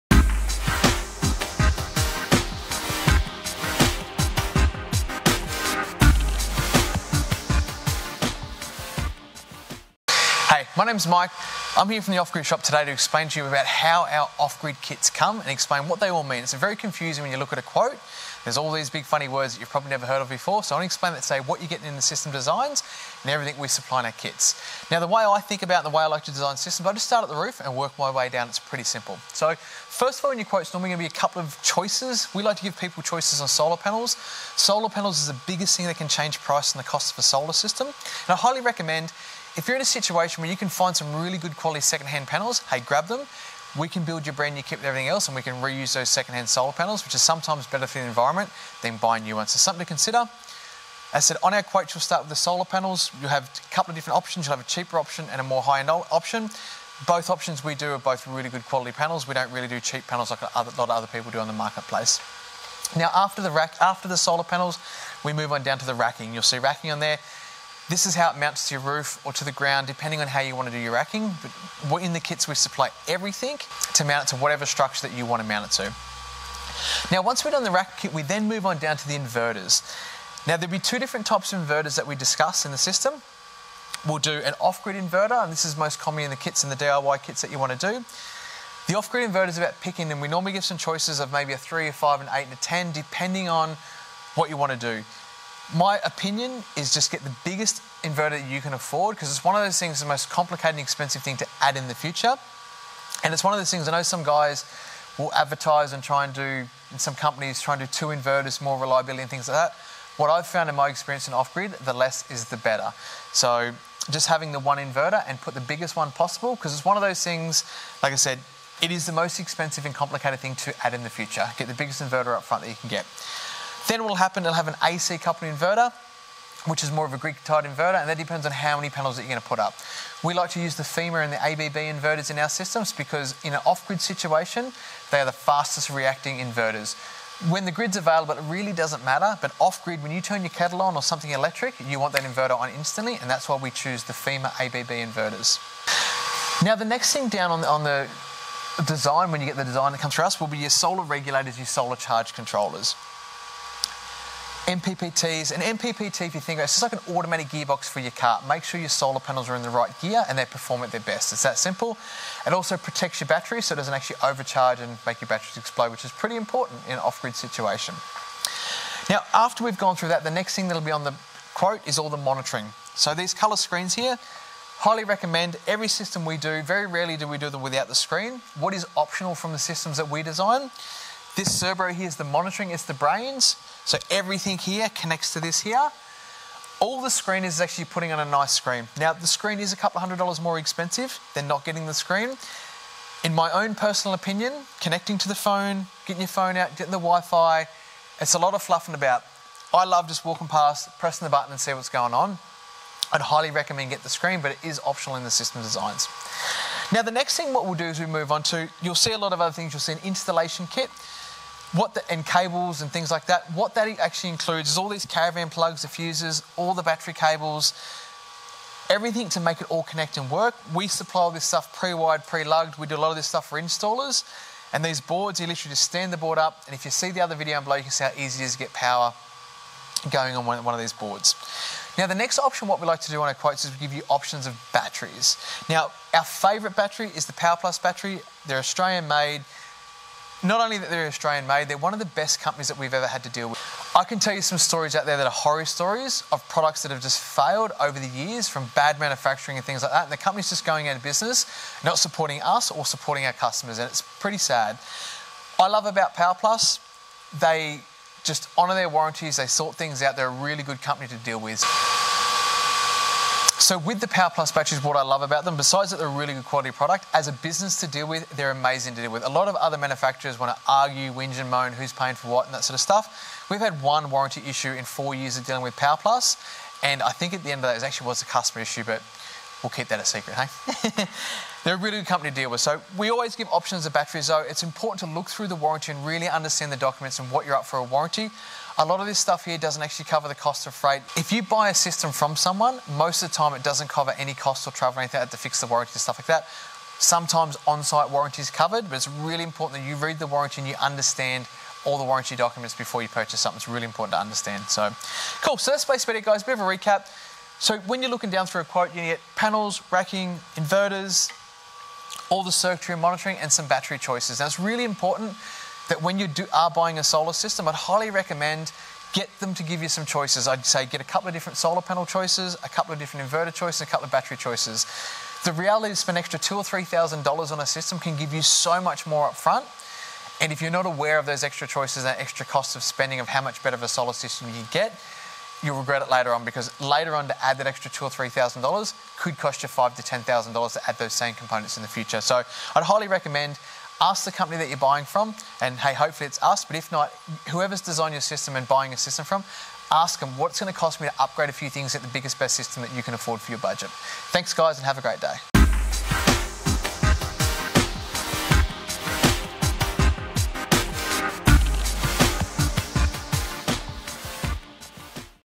Off Grid Solar | How to DIY in Australia | The Off-Grid Shop
If you are looking to go off-grid solar in Australia, this is a great video on how to DIY solar kits, which explains all the different components you'll need to consider when setting up your solar system.